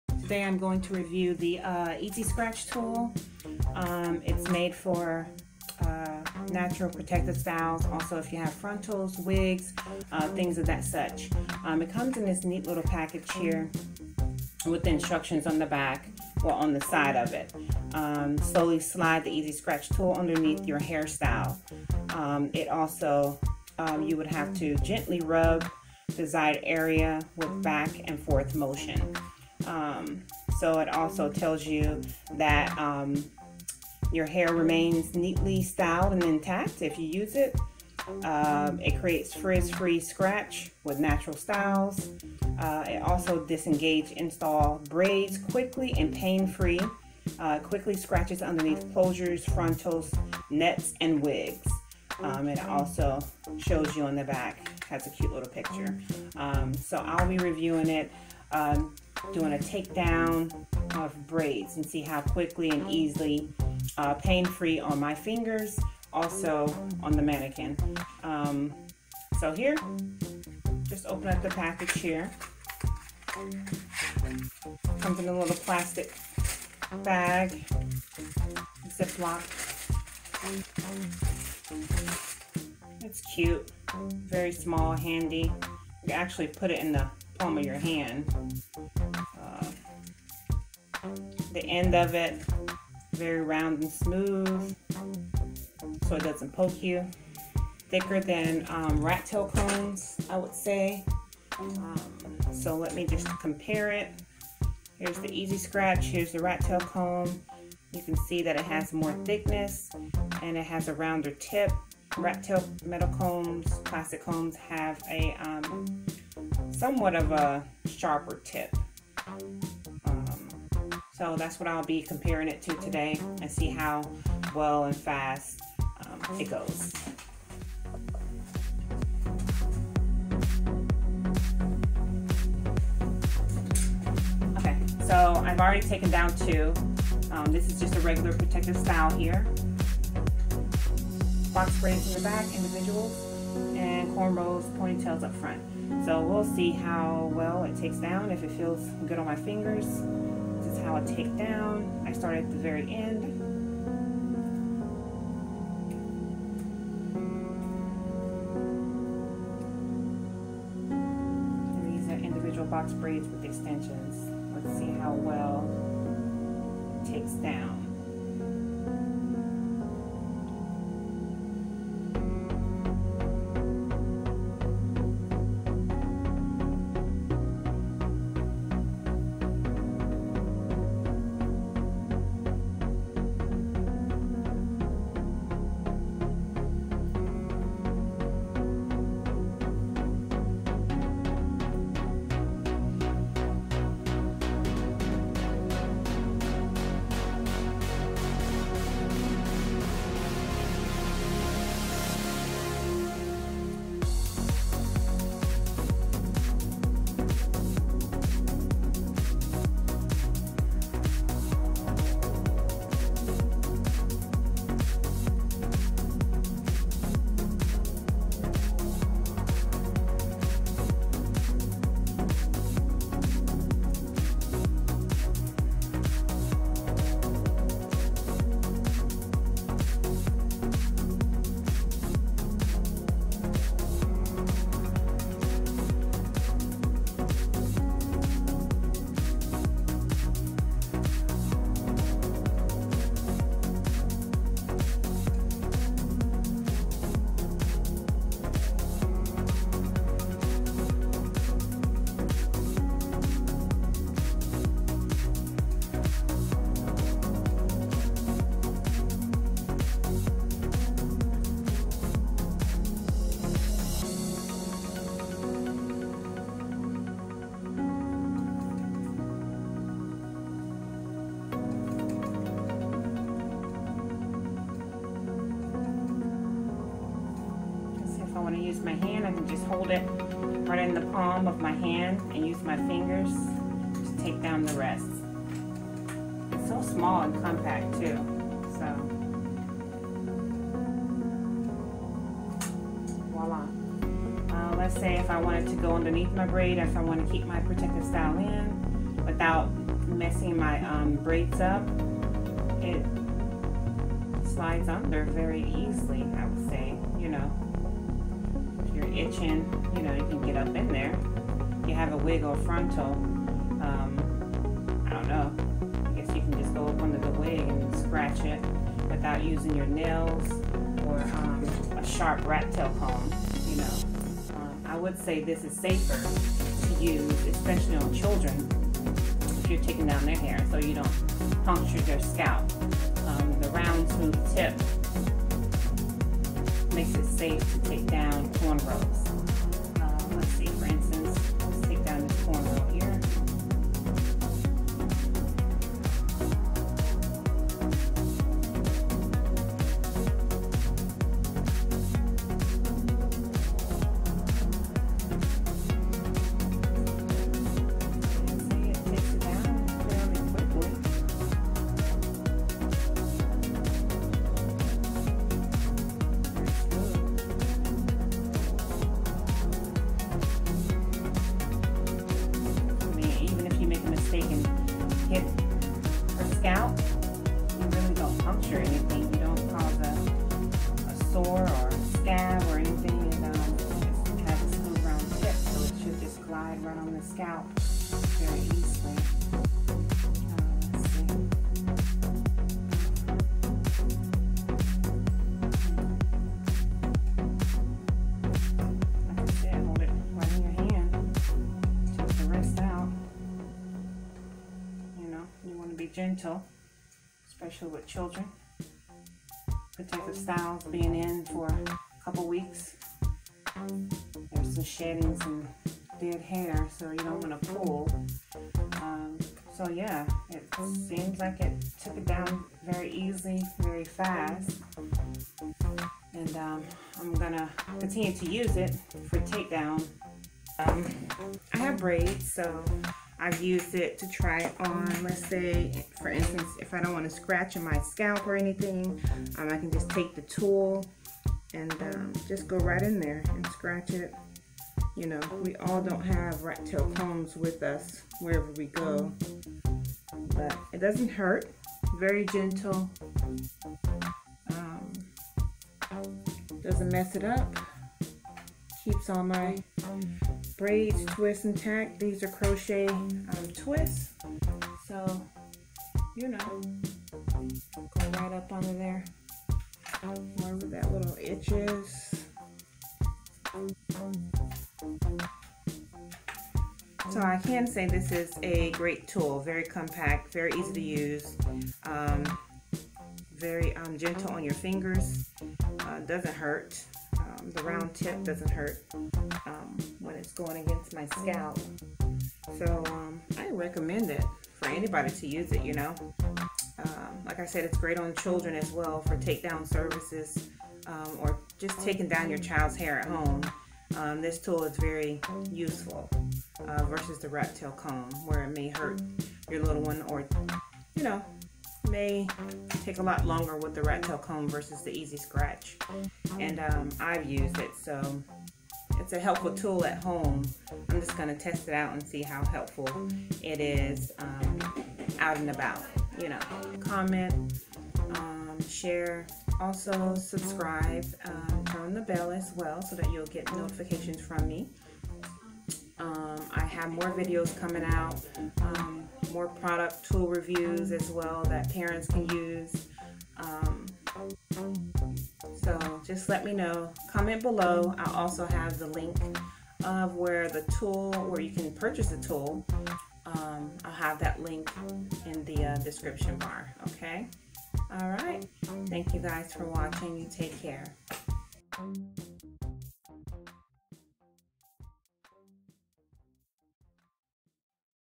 When to take your braids out
I test out the EzScratch Tool on braids to see how efficient it is in so many ways.

FACEBOOK: GLAMOUR KIDS BRAIDS